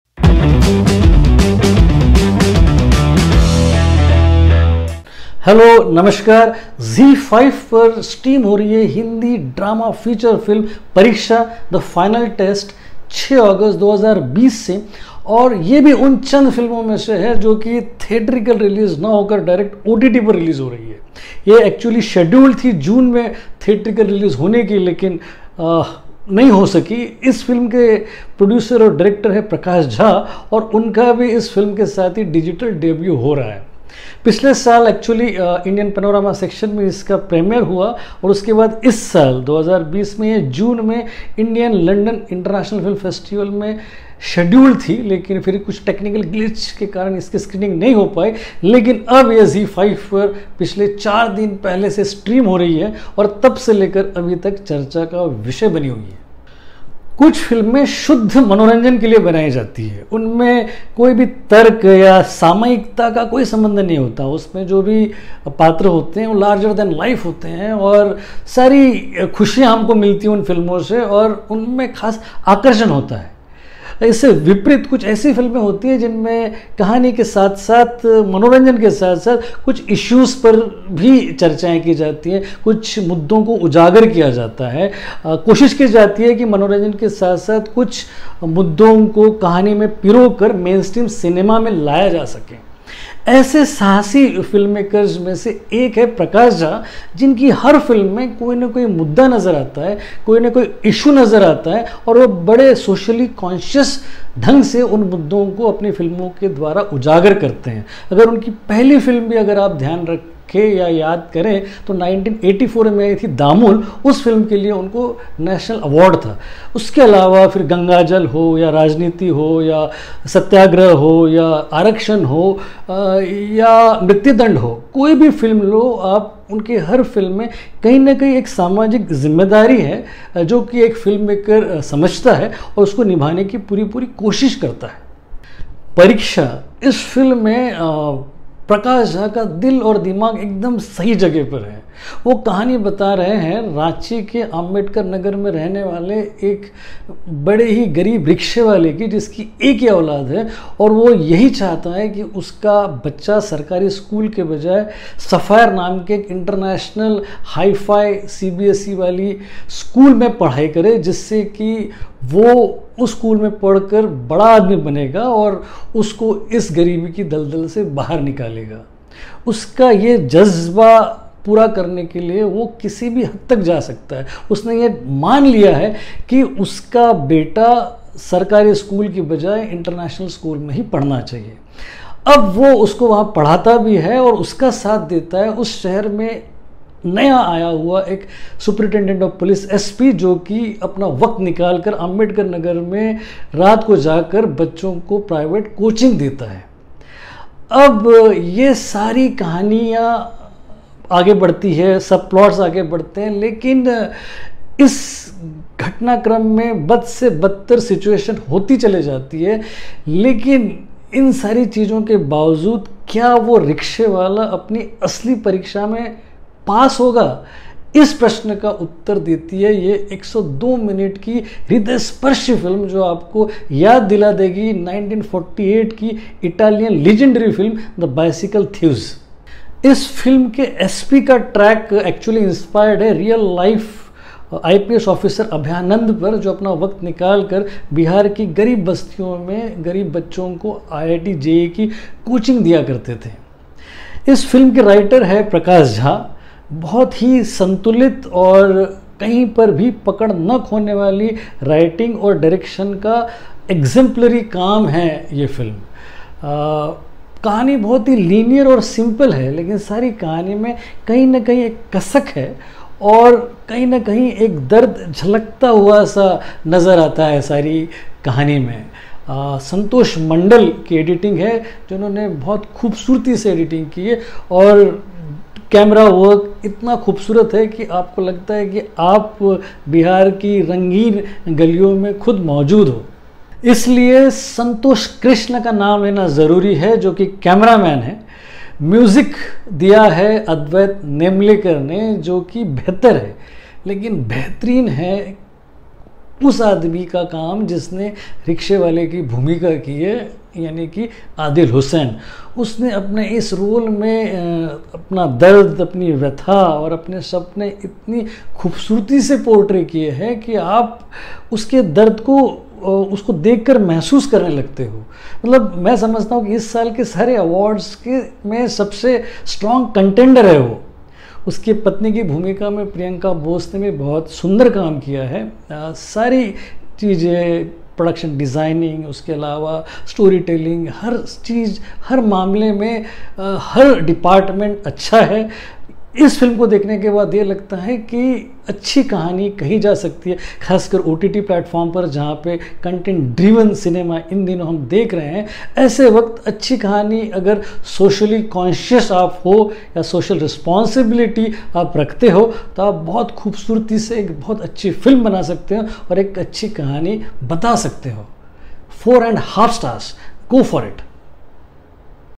Pareeksha Movie Review & Analysis | Prakash Jha Movie | Zee5 | Adil Hussain, Sanjay Suri
Priyanka Bose, Adil Hussain, Sanjay Suri
Pareeksha, a ZEE5 Original film directed by Prakash Jha. Inspired by real-life events, the film chronicles the story of a rickshaw puller who aspires to give his son the best possible education.